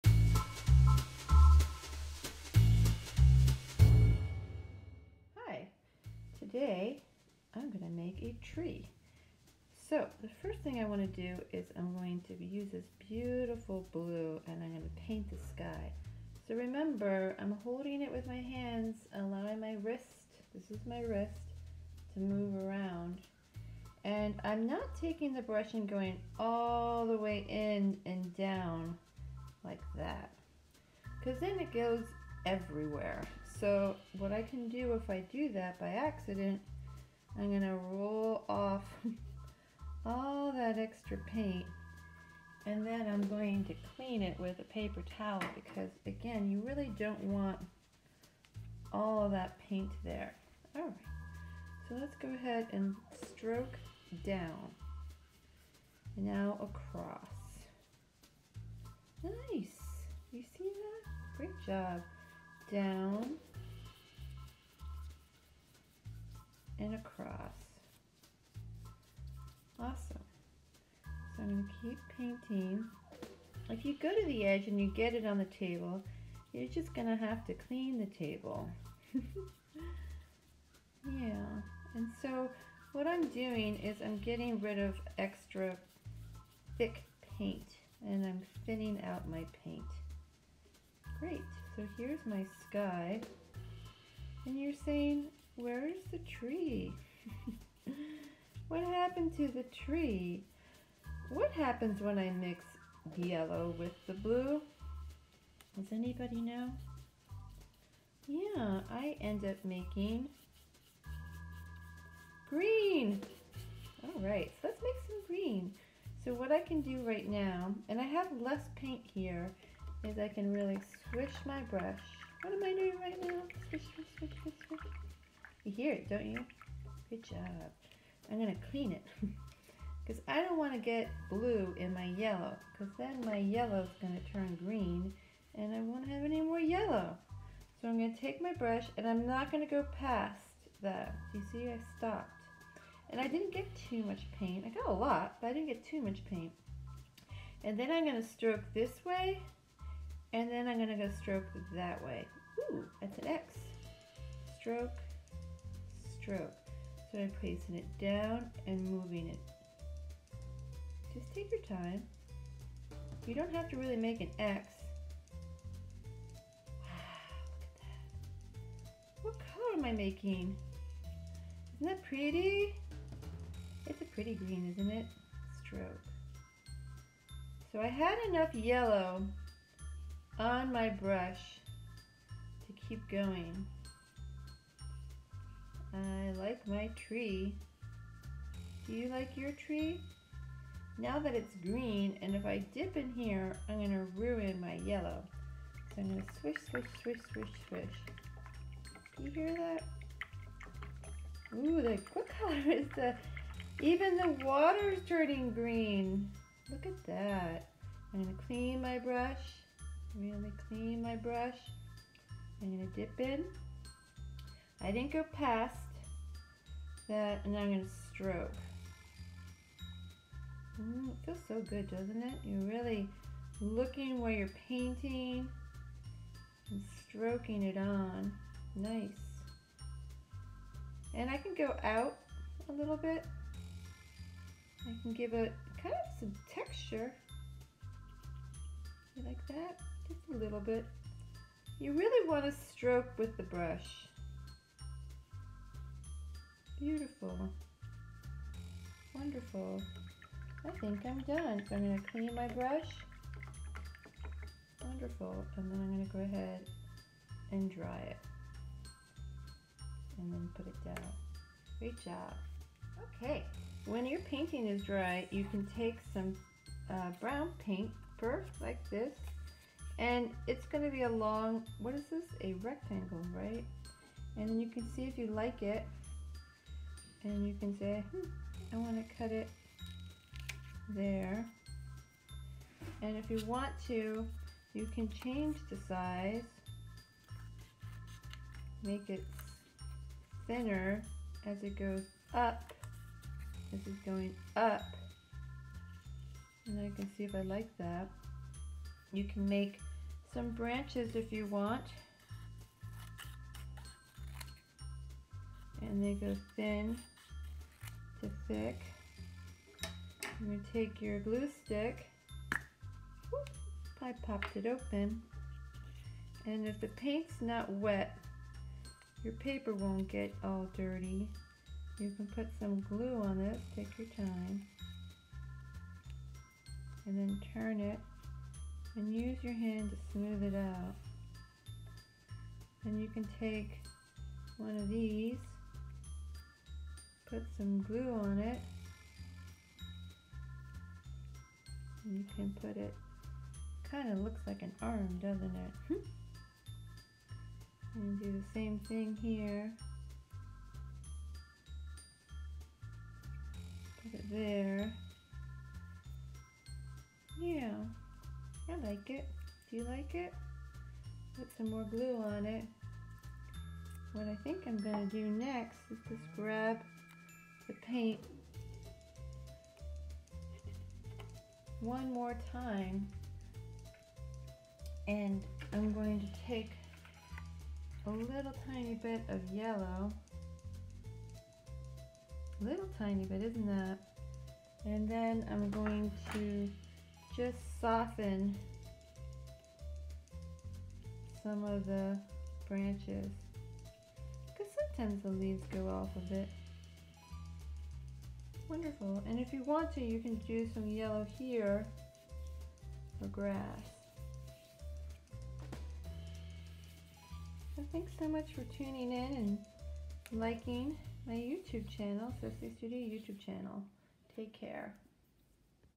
Painting- Tree for Kindergartners using Tempera Paint
In this video, I use tempera paint, brushes, an ice tray, white paper, a water container, brown paper, glue sticks and scissors. I talk about stroking the brush and the direction of the stroke. I add paper on the painting later.

K.1 The student will apply creative thinking to artmaking.
 a) Draw from imagination
 b) Create works of art that represent personal responses to art-making challenges.
K.12 The student will identify and use the following in works of art:
  a) Color—red, blue, yellow, green, orange, violet, brown, black, white.
b)  Line—straight/curved, thick/thin, long/short, up/down/across.
c)  Shape—circle, square, triangle, rectangle, oval.
d) Texture—visual, tactile.
K.13. The student will identify spatial relationships for composition.
a)  Identify spatial relationships—left, right, top, bottom, side, center, front, back, over, and under.
Display pictures of trees:
K.14 The student will create drawings from observation.